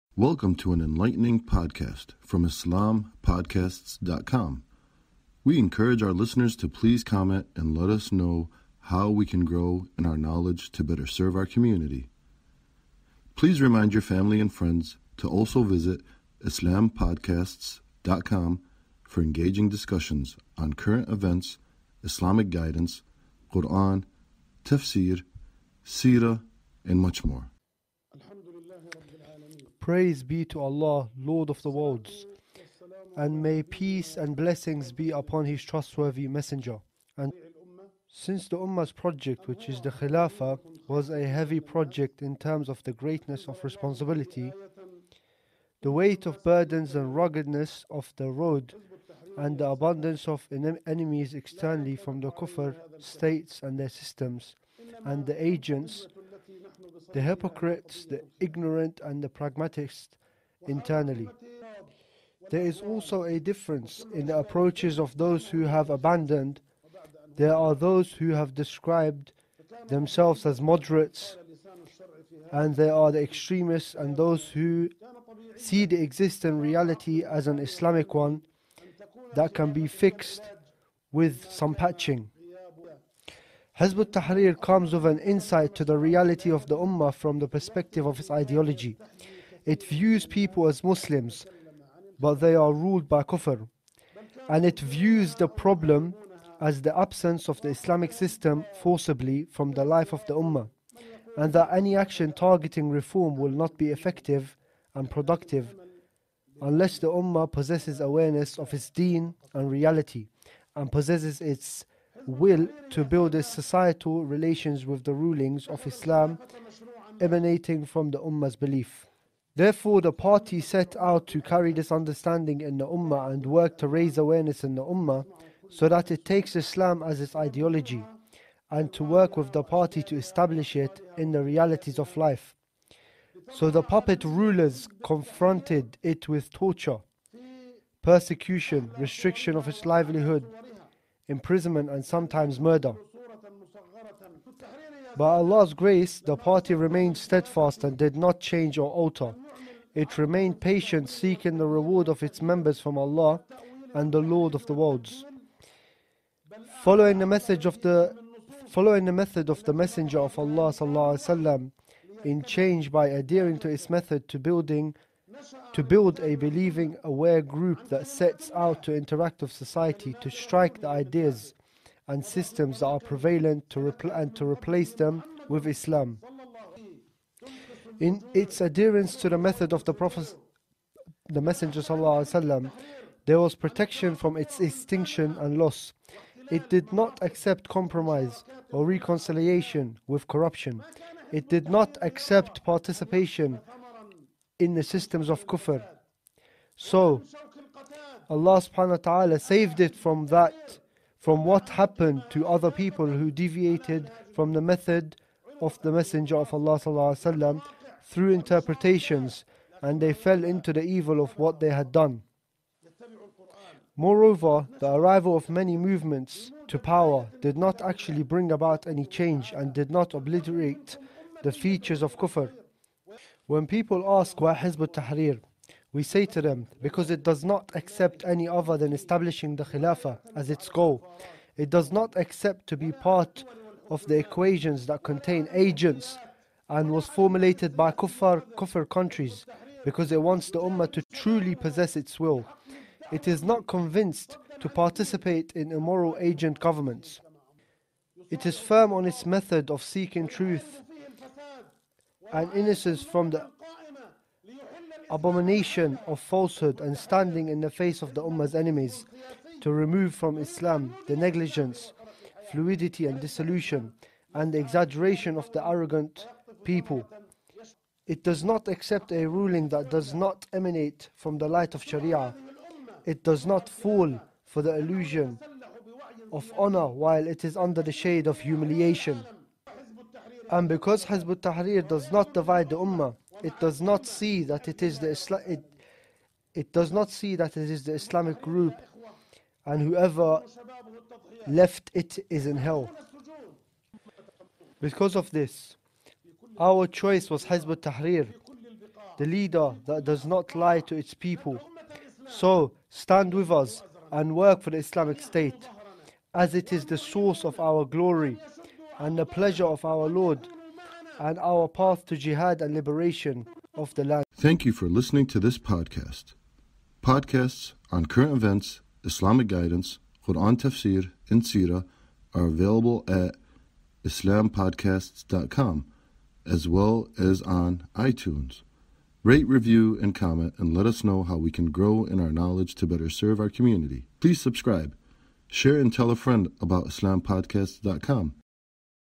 Why choose Hizb ut-Tahrir?
Many movements have come to power and have not actually brought about change and did not eliminate the symbols of kufr.

Join the talk on understanding a detailed answer when people ask why Hizb ut Tahrir believes it is the party that can revive the Islamic Ummah from the severe decline that it has reached, and to liberate it from the thoughts, systems and laws of Kufr, as well as the domination and influence of the Kufr states.

This talk is one talk in a series of ten enlightening talks from the Global Conference to the conclusion of the month long campaign "Upon the Centenary of the Destruction of the Khilafah... Establish it, O Muslims," for the month of Rajab 1442 AH.

Listen to the audio podcast of the concluding global conference for the activities of the campaign  launched by the Central Media Office of Hizb ut Tahrir under the direction of the Ameer of the Hizb, Ata'a bin Khalil Abu Al-Rashta, upon the occasion of the centenary of the demolition of the Khilafah.

Conference was broadcast live on the evening 03/13/2021, corresponding to Rajab 29 Muharram 1442 AH at 20:00​ Madinah time on Al​-Waqiyah Channel.